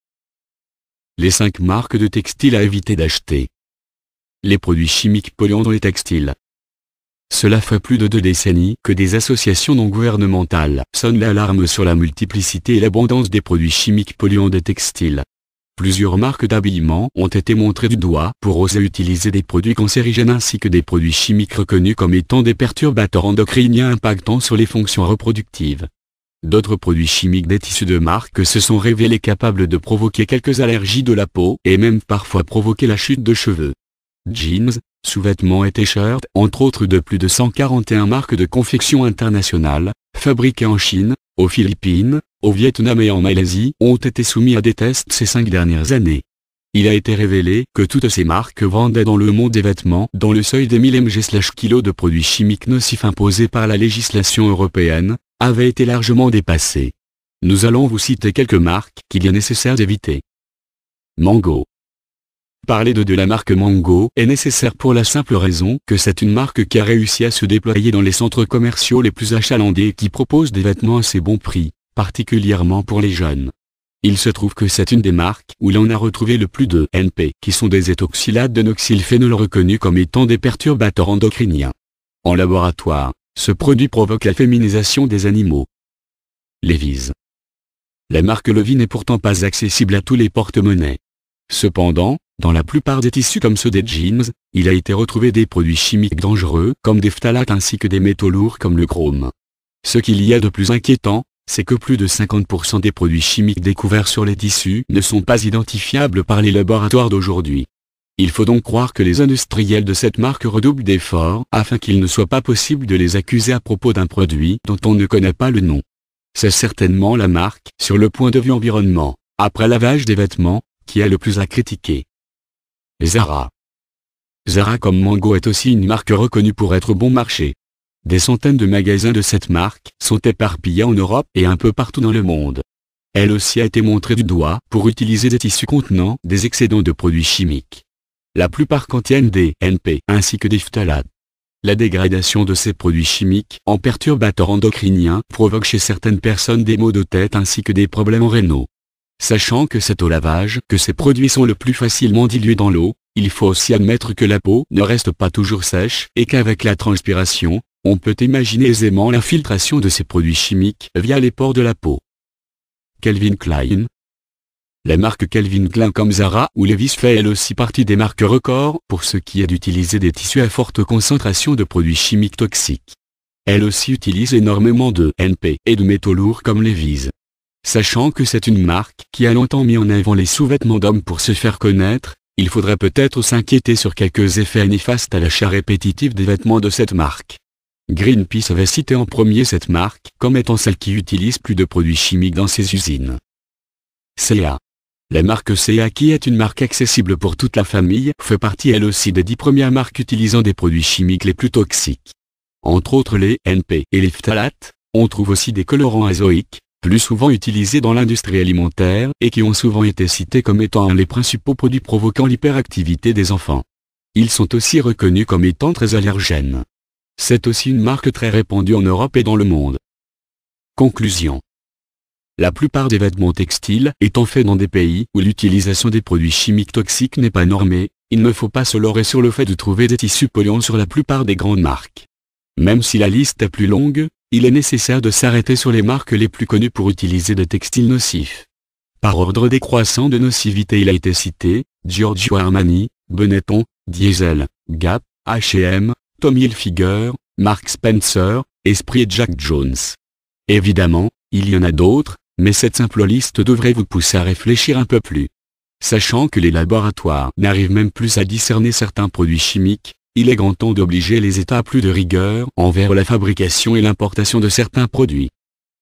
MODE : 5 marques utilisant des colorants chimiques dangereux
Cela fait plus de deux décennies que des associations non-gouvernementales sonnent l’alarme sur la multiplicité et l’abondance des produits chimiques polluants des textiles. Plusieurs marques d’habillement ont été montrées du doigt pour oser utiliser des produits cancérigènes ainsi que des produits chimiques reconnus comme étant des perturbateurs endocriniens impactant sur les fonctions reproductives. D’autres produits chimiques des tissus de marque se sont révélés capables de provoquer quelques allergies de la peau et même parfois provoquer la chute de cheveux.